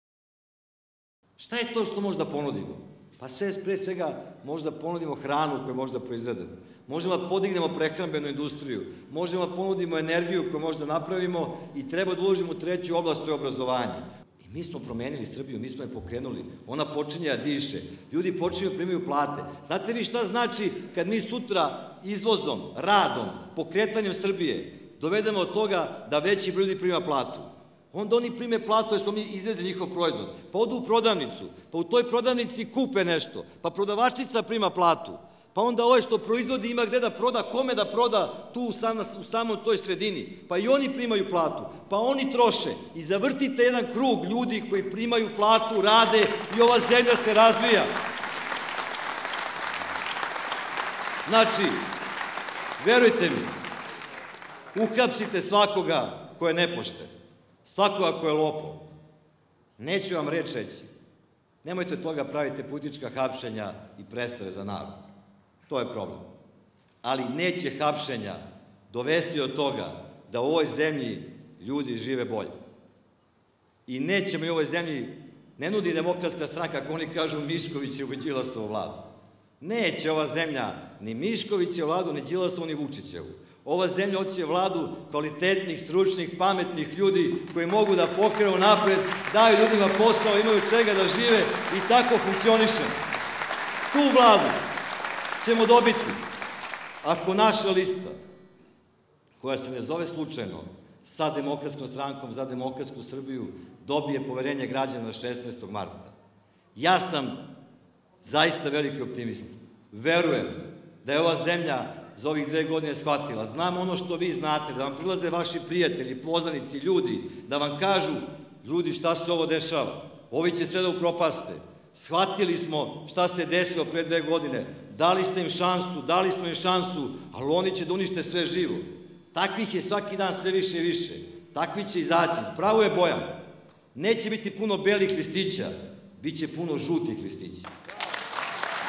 Dragan Đilas: Da Srbija pocne da diše, a ljudi da žive
(Zrenjanin, 19. februara 2014) Konvencijom u Zrenjaninu lista "Sa Demokratskom strankom za demokratsku Srbiju" nastavila je kampanju za vanredne parlamentarne izbore.

Predsednik Demokratske stranke Dragan Đilas poručio je prisutnima u punoj sali Kulturnog centra da danas živimo u zemlji koja počiva na obmanama i lažima.

"6. januara Aleksandar Vučić je rekao da je stopa nezaposlenosti pala na 21 posto. Mi smo tada rekli da je to nemoguće, zbog čega smo bili napadani. Prekjuče je Republički zavod za statistiku saopštio da je stopa nezaposlenosti 29 posto. Razlog takvog ishoda je što niko u državi nema ideju ni viziju kako da zaposlimo ljude", rekao je Đilas.

Lider DS je poručio da jedino Demokratska stranka ima plan za pokretanje Srbije.

"Rešenje nije da smanjimo plate lekarima, sudijama i profesorima jer to ne znači da će oni koji žive od 20.000 dinara imati veće plate, već da ulažemo u prehrambenu industriju, energetiku i obrazovanje. Ako to budemo uradili, Srbija će prodisati. Podići ćemo izvoz a samim tim stimulisati i ostale industrijske grane", rekao je Đilas i podsetio da je zrenjaninski Šinvoz jedini pravi posao dobio od grada Beograda.

Đilas je pozvao Aleksandra Vučića da dođe u jednu televizijsku emisiju sa predstavnicima opozicije i konačno kaže na koji način želi da ekonomski spase Srbiju, a ne samo da priča kako će to uraditi, bez iznošenja ikakvog plana.

"DS je sada opet ona ista stranka koju je pravio Zoran Đinđić sa ciljem da se u Srbiji normalno i pristojno živi. To ljudi sve više prepoznaju i sa ponosom će u velikom broju glasati za našu listu", zaključio je Dragan Đilas. (za više informacija posetite